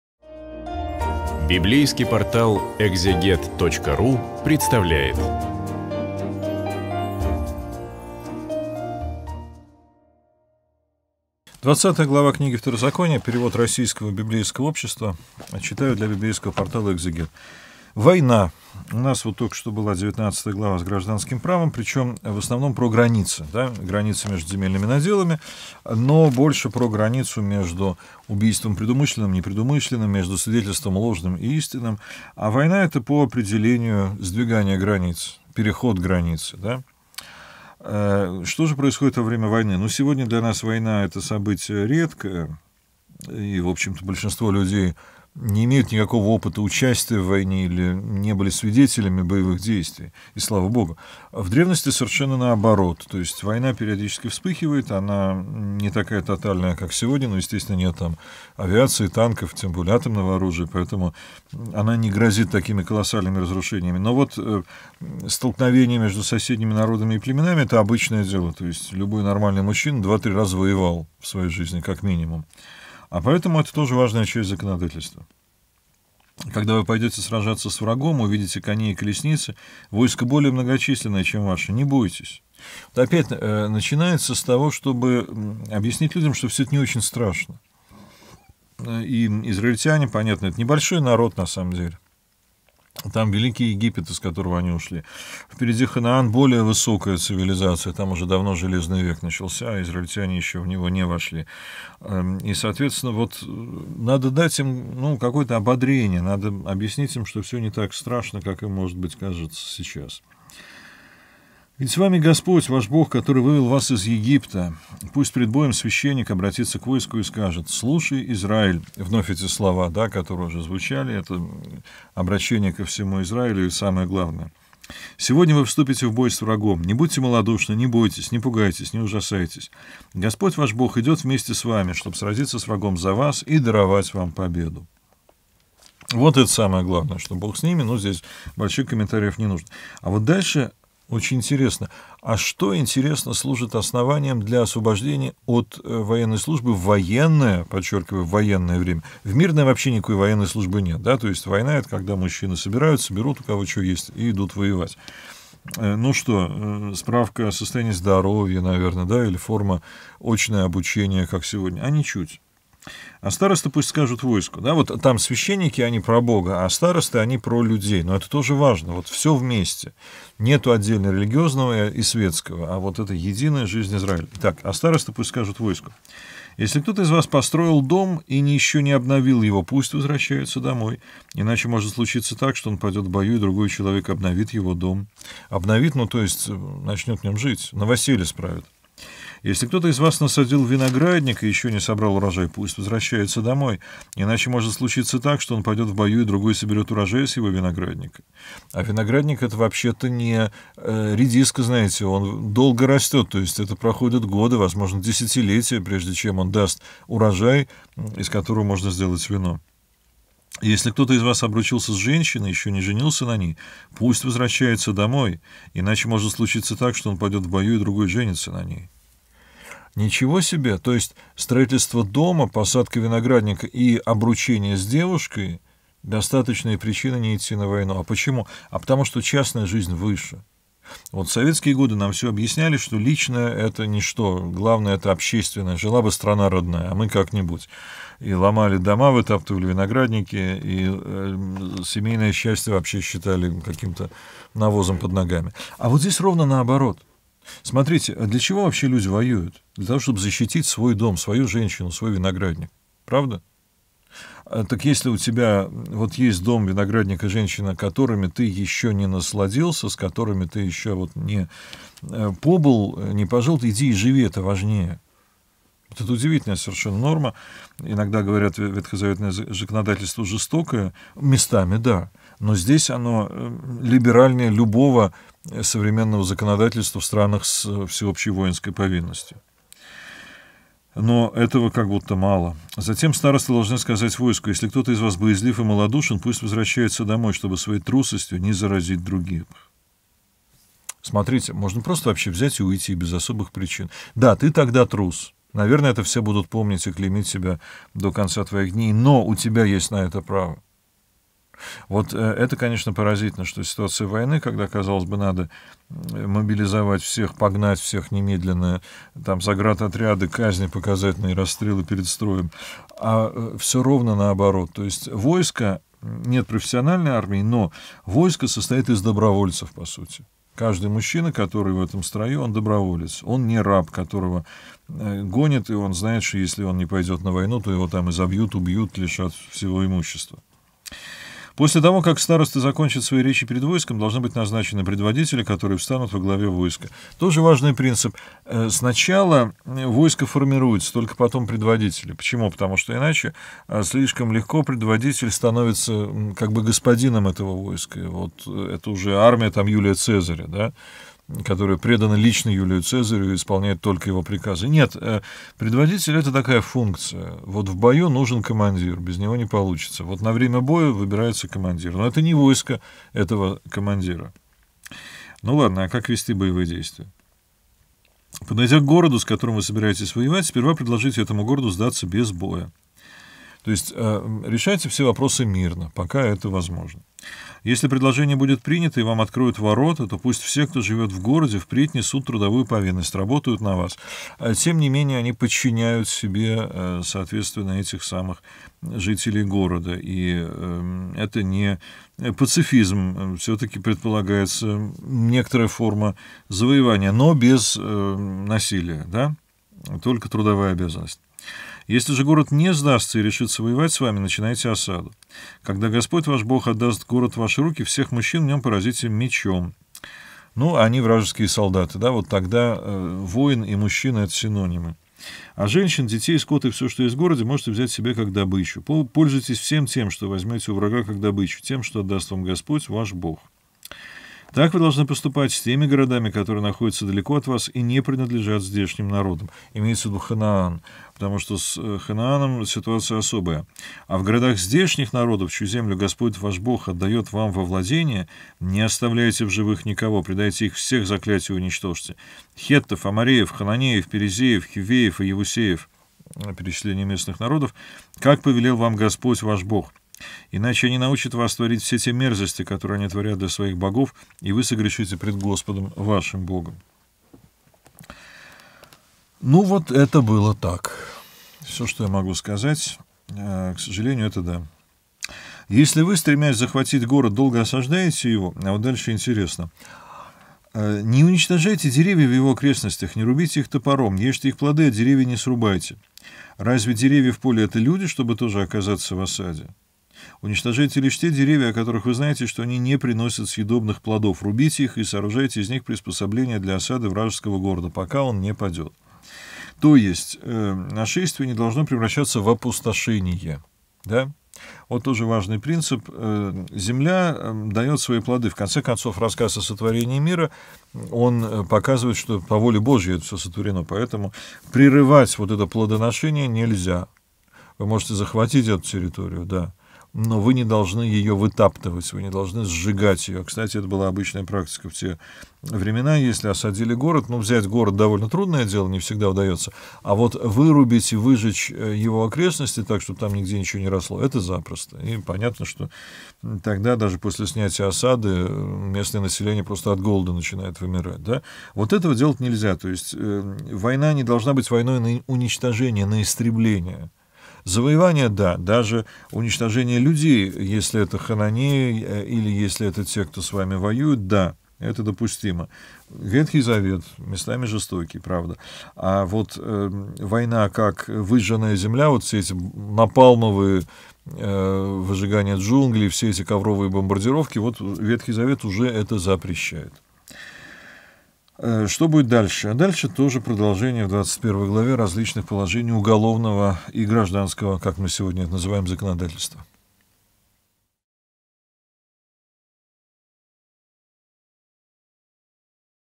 Второзаконие. Глава 20. Андрей Десницкий. Библия
Продолжаем цикл лекций на Второзаконие, профессора Андрея Десницкого. Сегодня разбираем 20-ю главу (перевод РБО).
Инструкция в военное время.
Не нужно боятся боевых столкновений, страх мешает человеку сделать верное решение.
Перед боем священник должен обратиться с молитвой к Богу, а потом к своему народу и дать напутствие перед битвой.
Причины, по которым воин может быть временно отстранен от военной службы, это не достроенный дом, не собранный урожай винограда, обрученные пары, отсутствие боевого духа.
Староста выбирает предводителя, когда уже войско собранно.
Сначала нужно вести переговоры прежде чем вступать в битву.
Если можно выиграть войну без кровопролития, то это самое лучшее решение.
В добычу у врага можно брать все — скот, женщин, имущество.